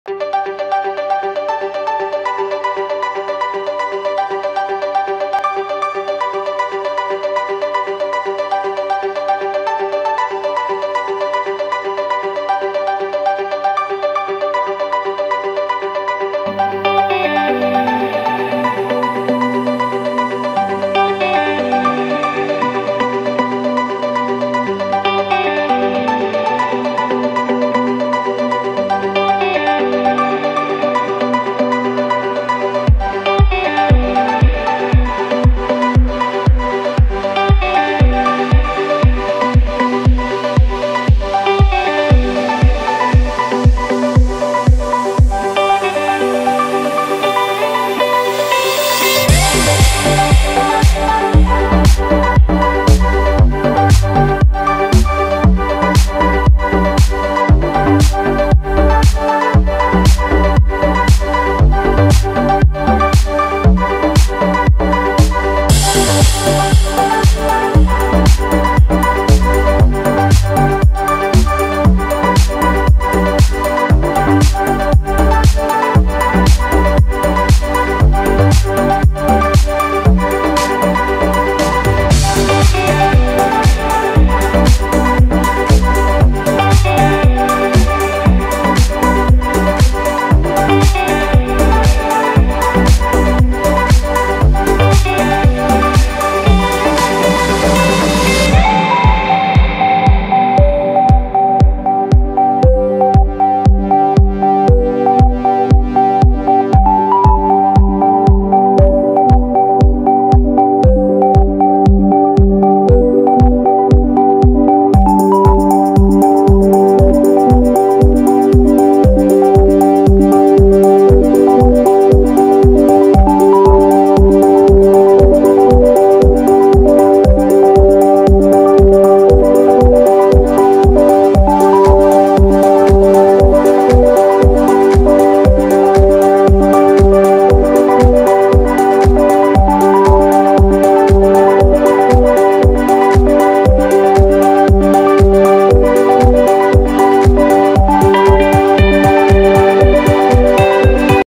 france ,nice ,
City tour of Nice, . Its natural picturesque beauty in combination with lively city.Cap a perfect day in Nice with a dip in the ocean and a walk along the seashore, watching the sun disappear into the sea